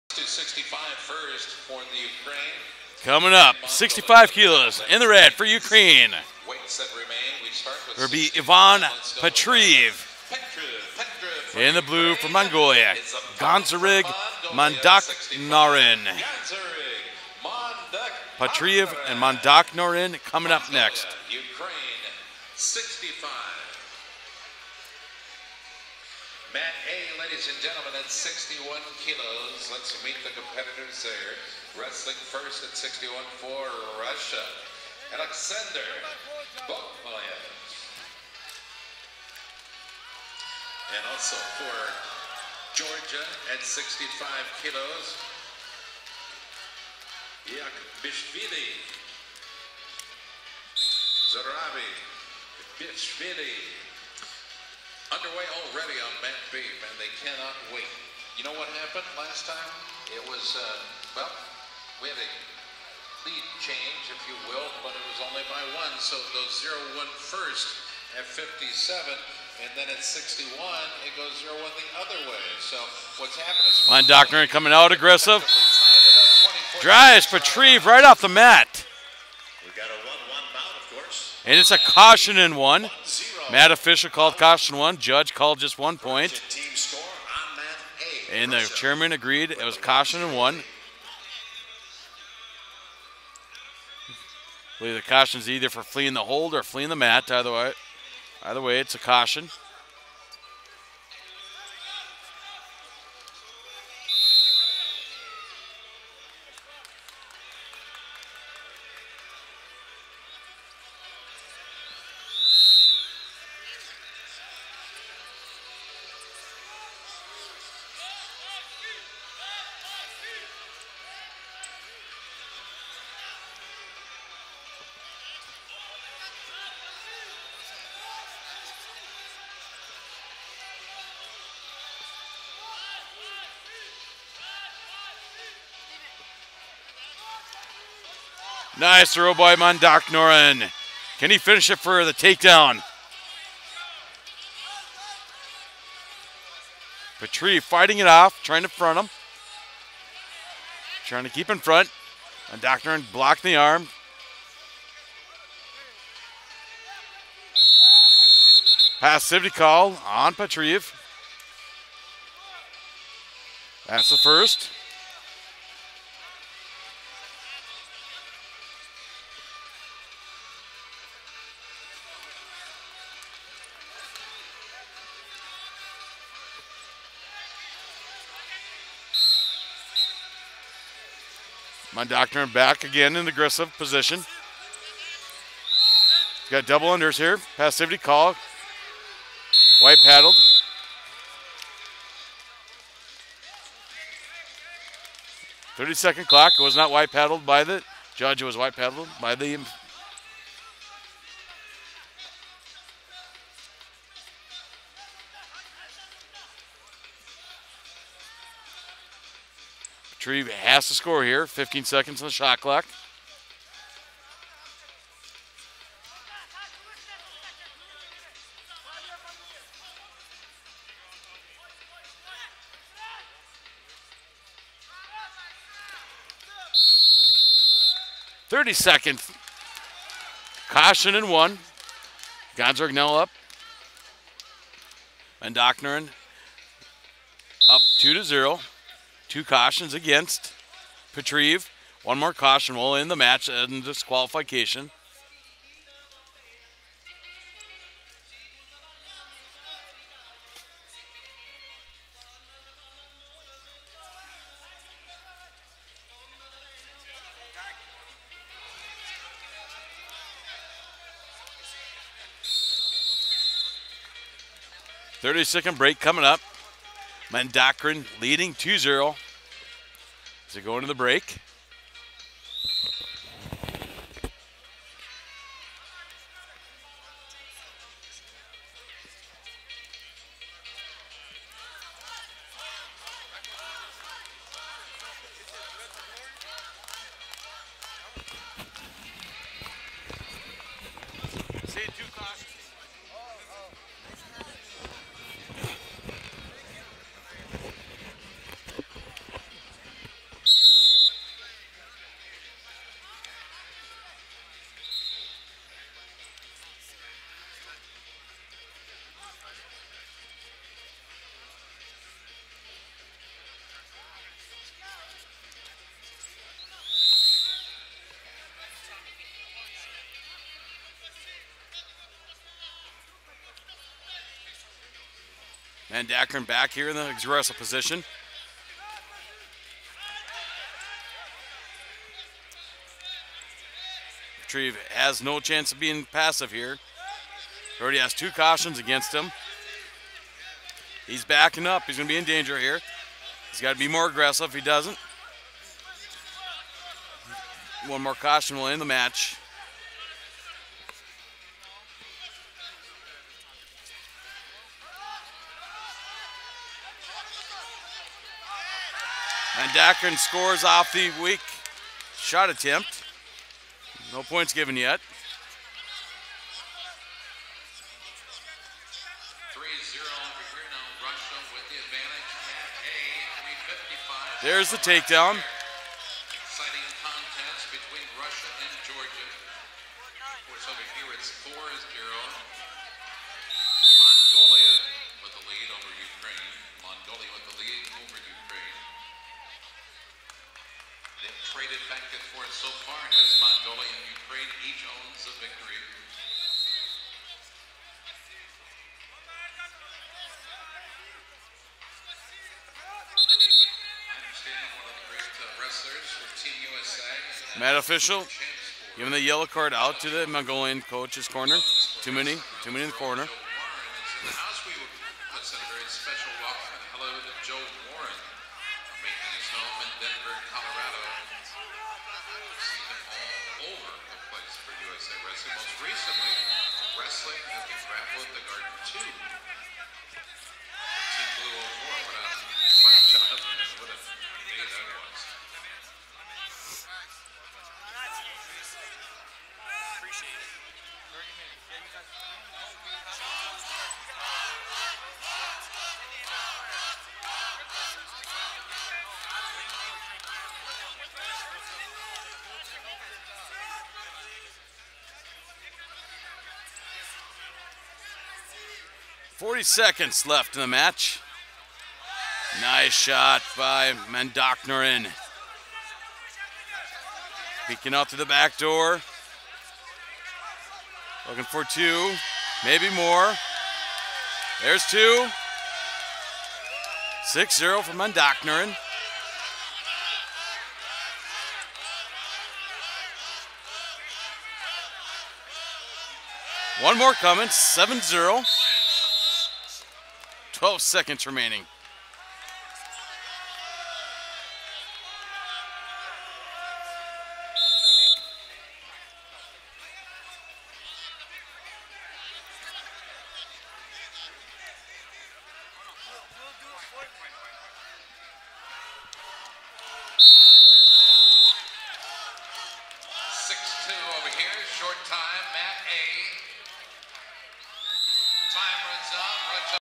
65 KG Ivan Petriv Ukraine vs Ganzorig Mandakhnaran Mongolia
65 KG Ivan Petriv Ukraine vs Ganzorig Mandakhnaran Mongolia - 2014 World Cup